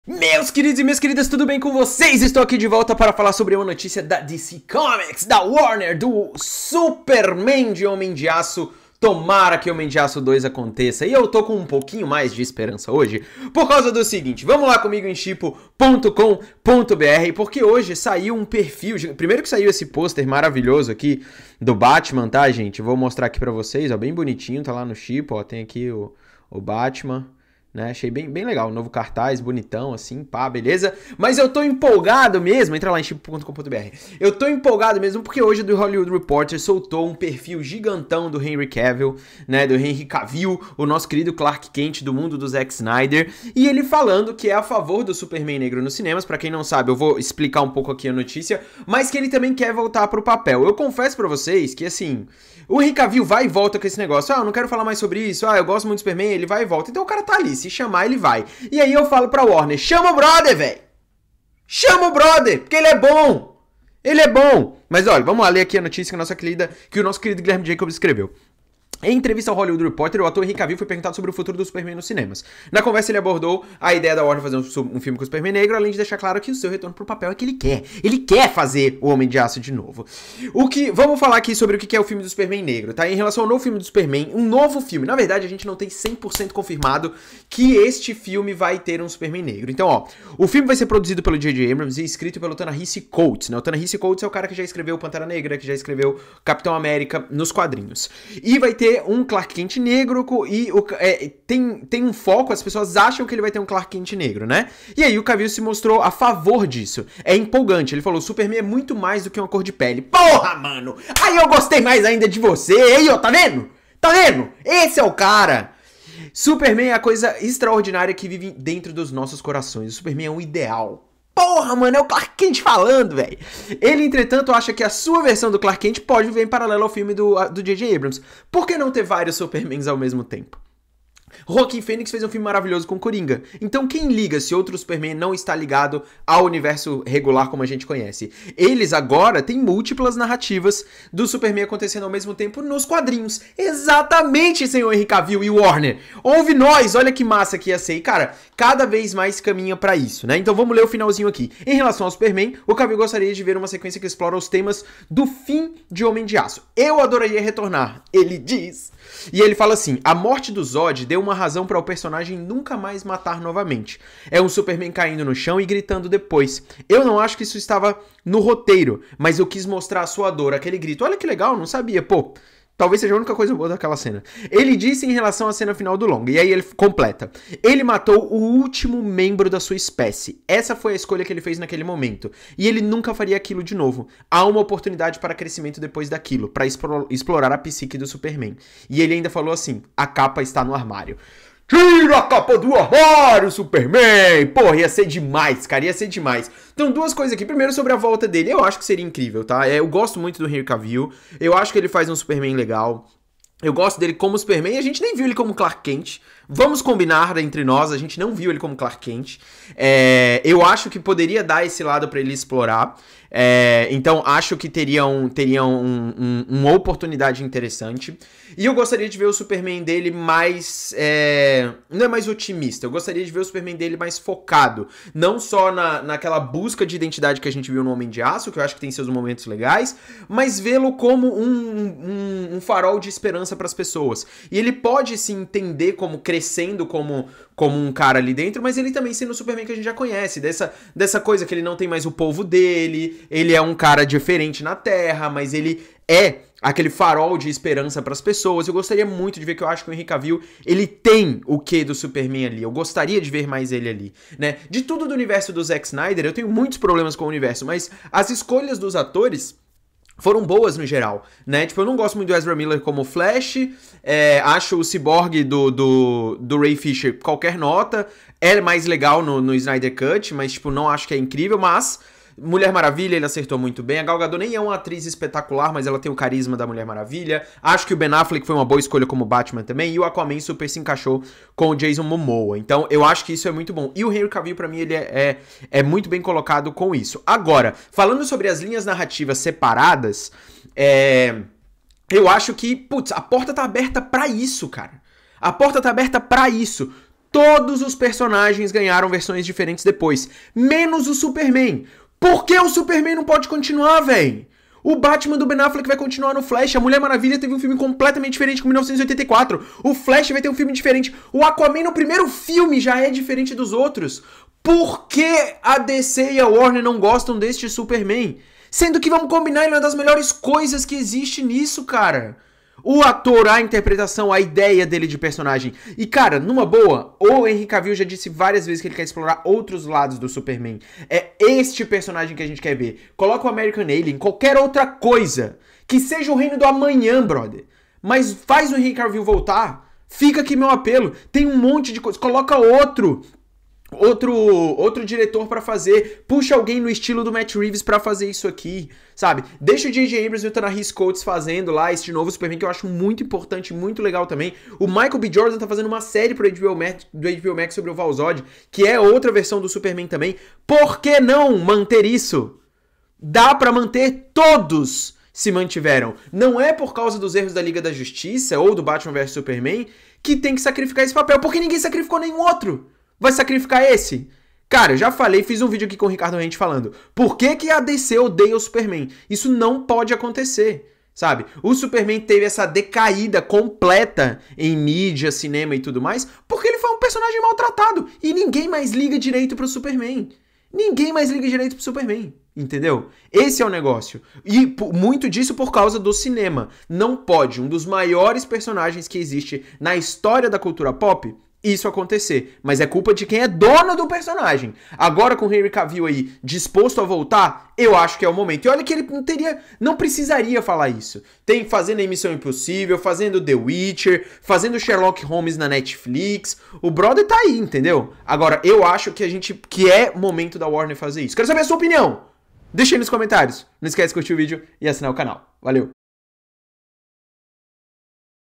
HOMEM DE AÇO 2! HENRY CAVILL DE VOLTA?
Henry Cavill, o Superman do Universo DC, está meio que aposentado do papel mas parece que as coisas podem mudar. Neste vídeo, mostramos a recente entrevista que o ator deu falando que gostaria de voltar a vestir a capa de Kal-El. E aí, o que acha? Comente!